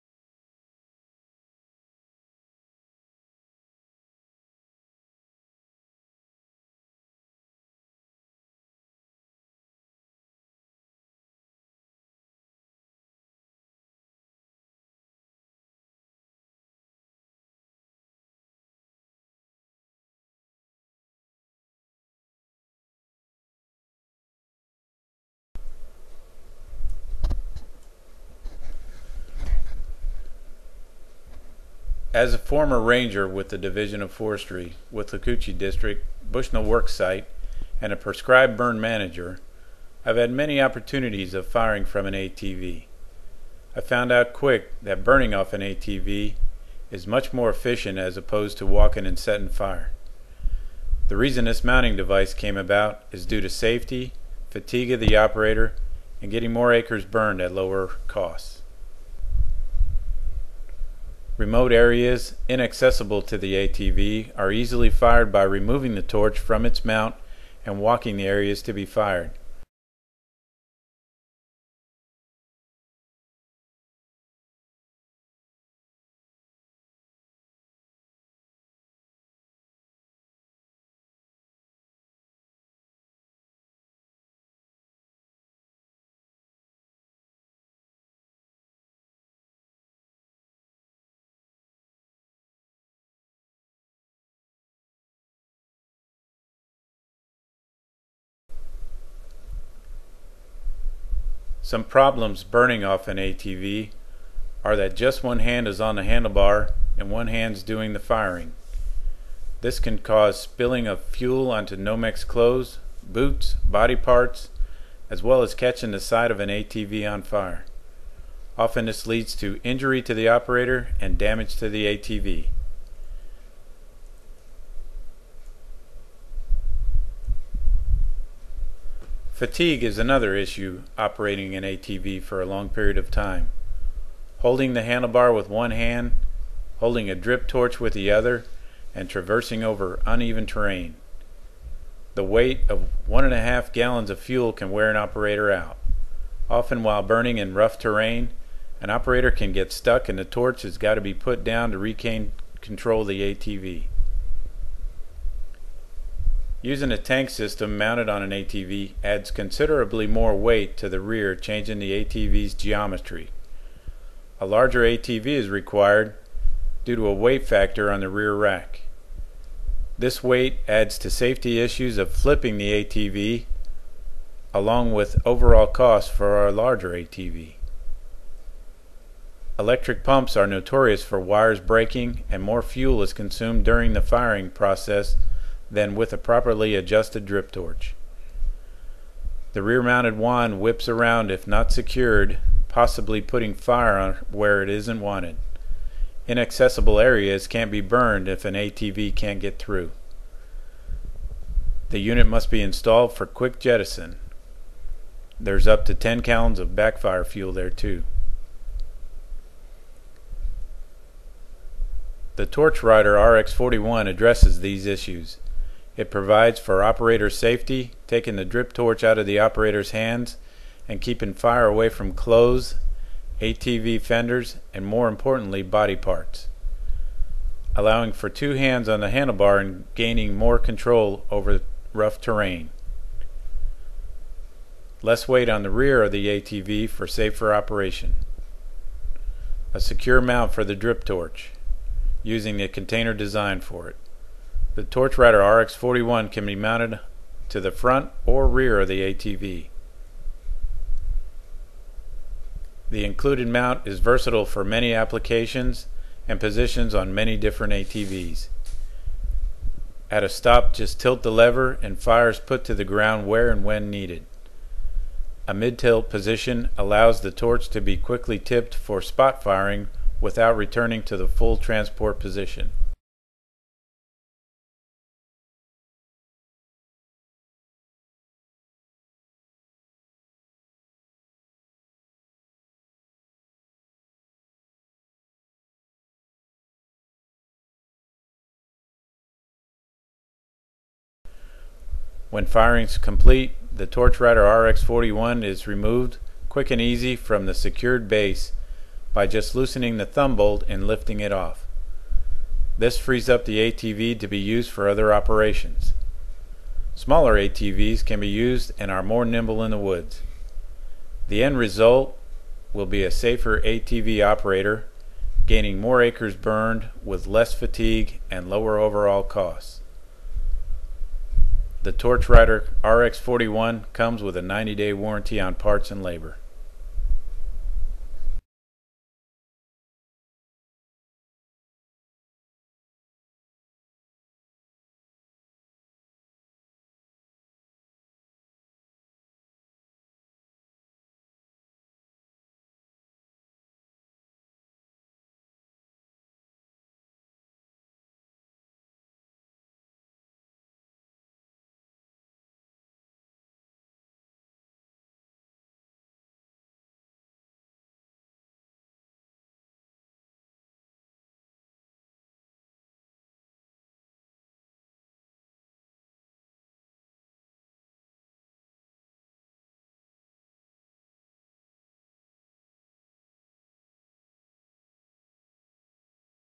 Rx Burn Video-take 5.wmv
The TorchRider is an amazing tool for prescribed burning. This new appliance allows firefighters to safely, efficiently, and cost effectively burn from an ATV.